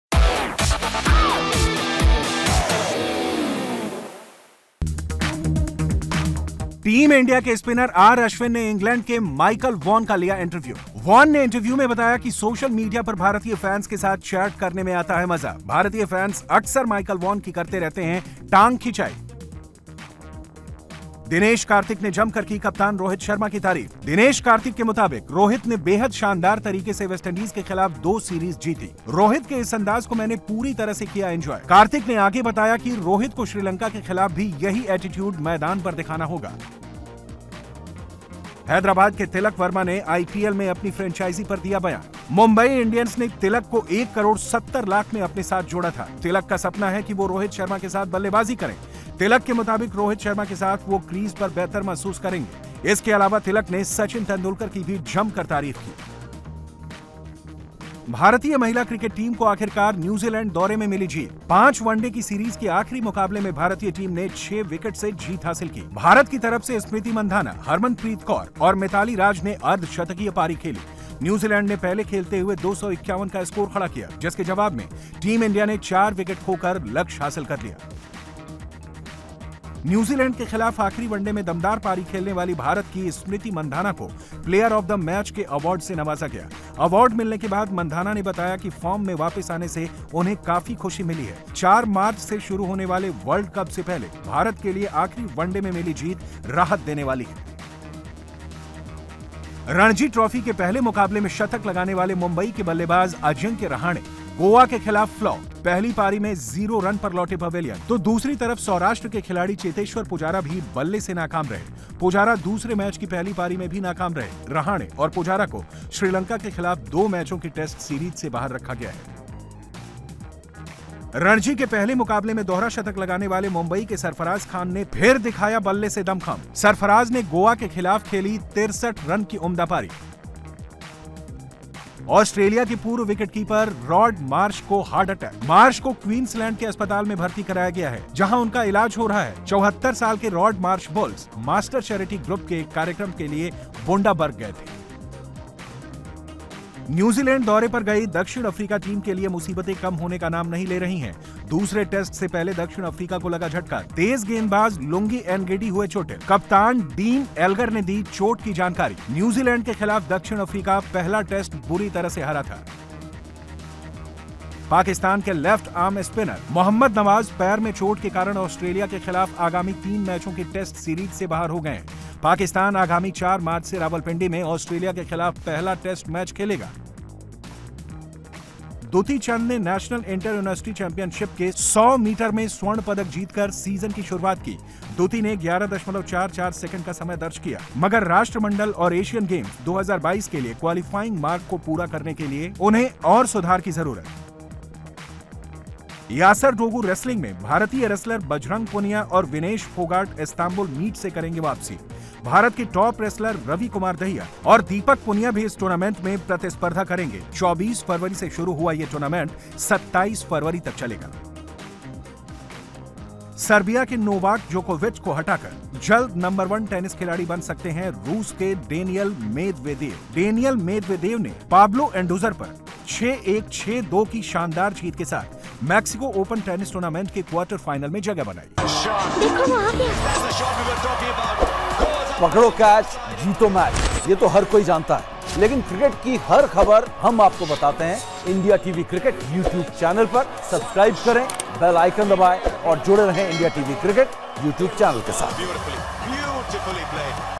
Rohit Sharma पर Dinesh Kartik का यू टर्न | Pujara-Rahane कुछ तो करो | Bajrang लाएंगे मेडल
Sports Fatafat में मिलेंगी आपको खेल के मैदान की सभी खबरें। India vs Sri Lanka के बीच आज से टी20 सीरीज का आगाज होने जा रहा है।